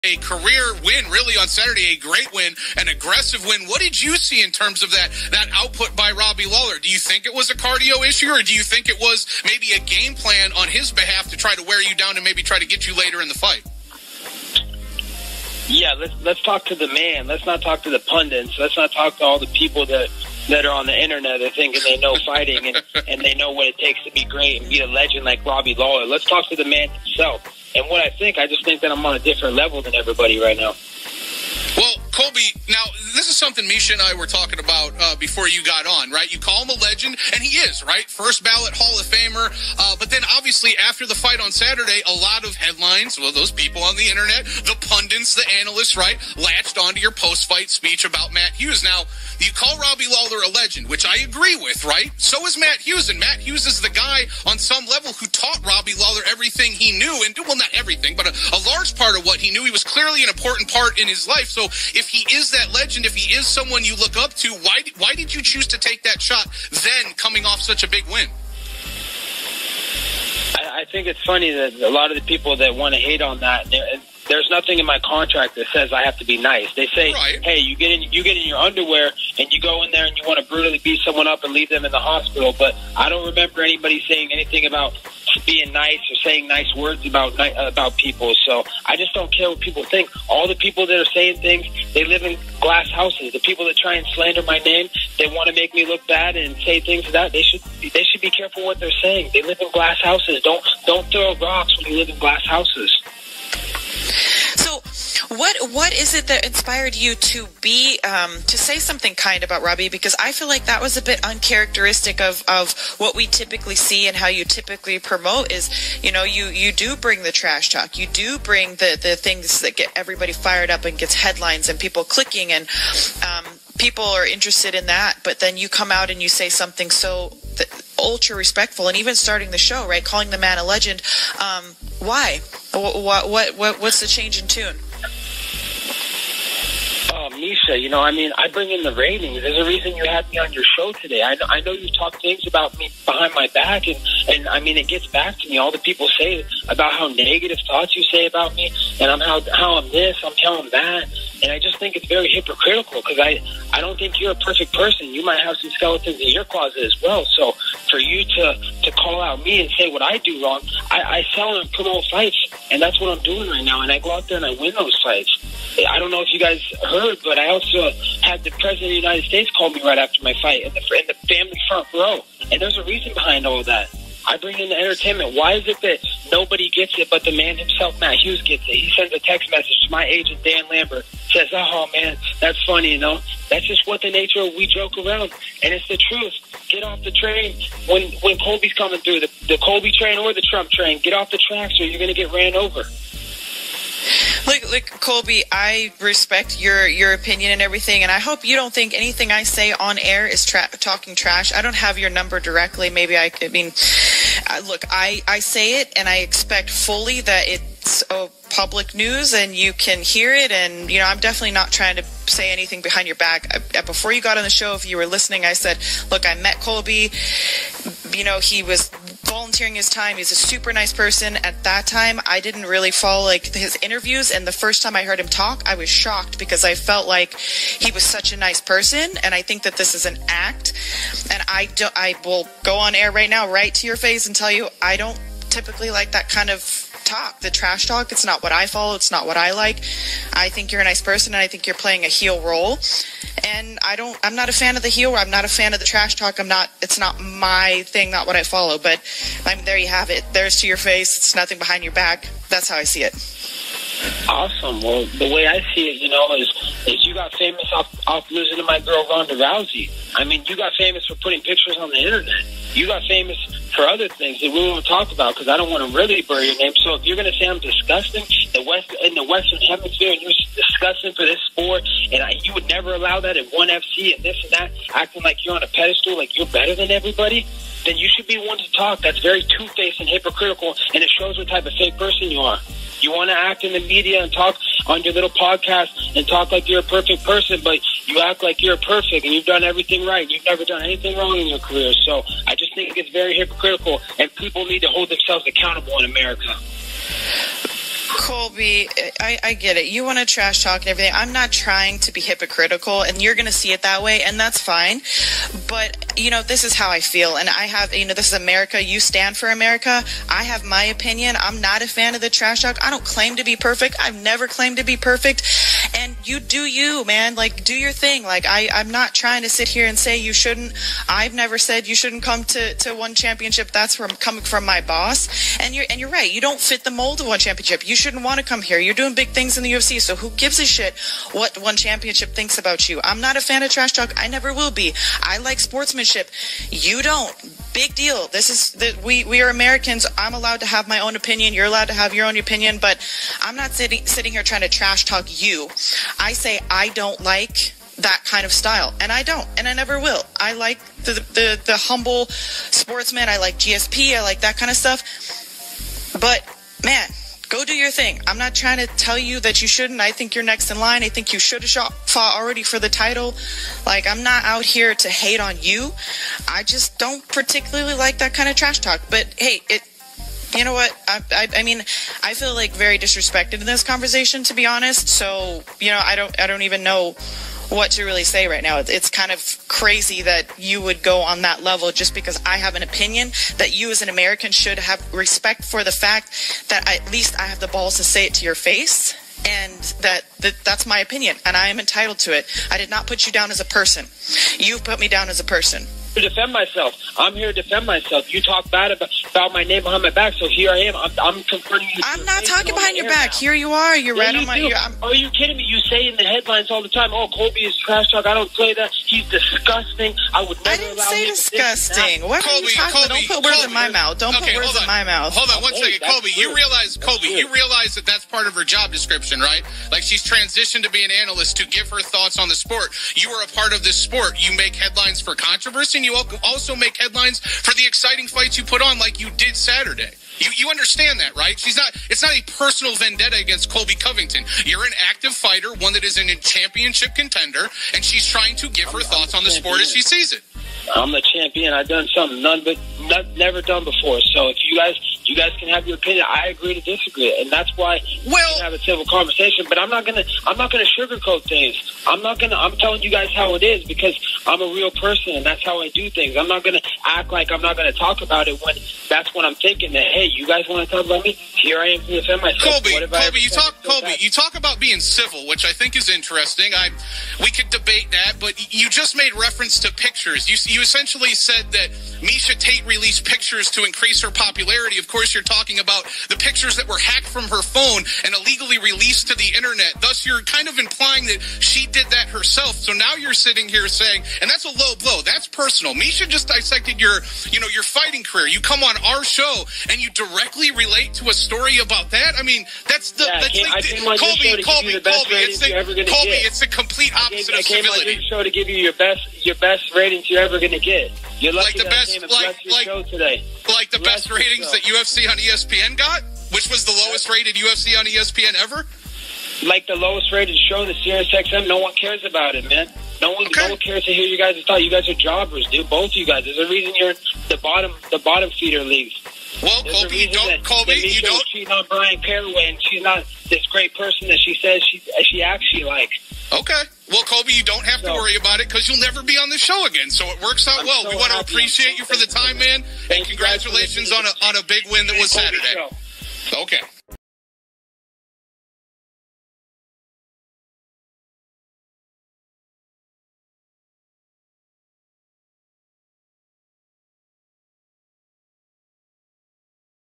Colby Covington's full leaked audio of controversial interview with Miesha Tate
Colby Covington's controversial interview with Miesha Tate where he was banned from the show later.